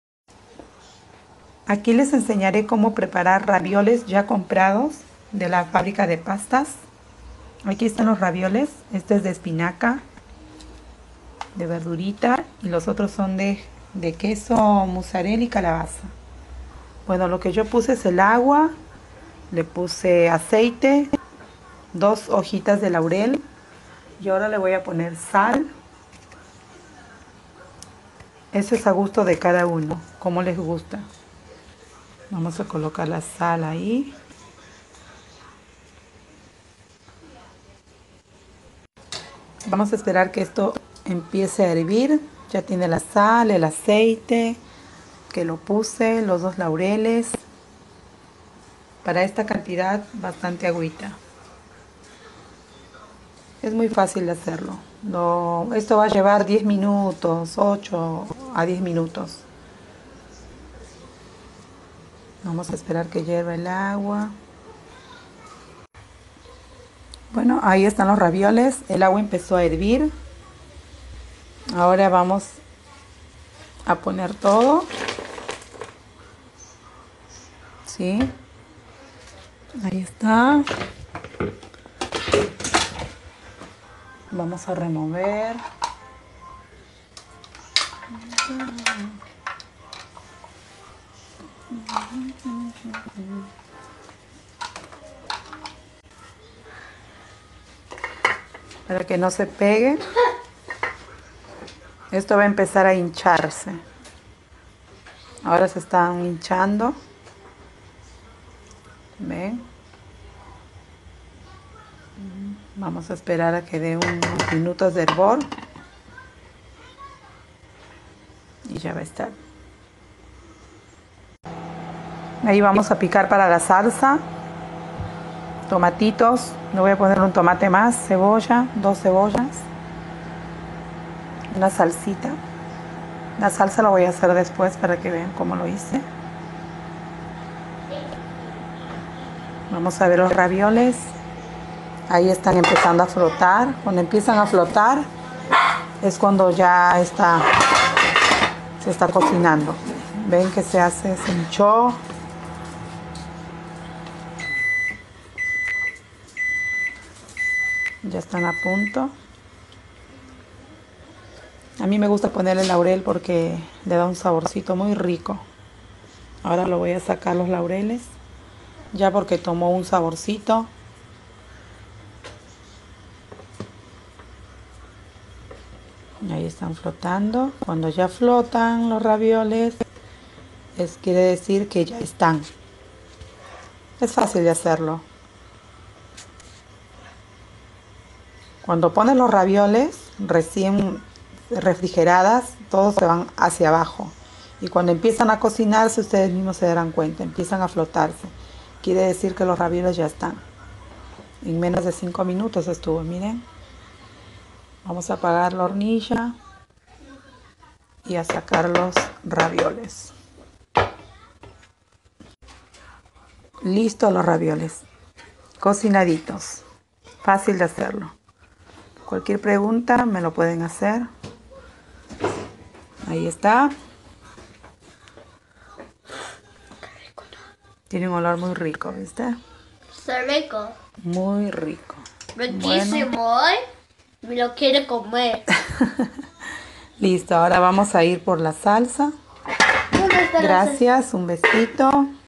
Como preparar Ravioles comprados
1/2 kg de Ravioles
2 hojas de laurel
Sal c/n
Aceite un chorrito.
Cuando empiezan a flotar los Ravioles es porque esta listo!
LES INVITO A SUPSCRIBIRSE ASI ME MOTIVAN A SEGUIR BAJANDO MAD VIDEOS!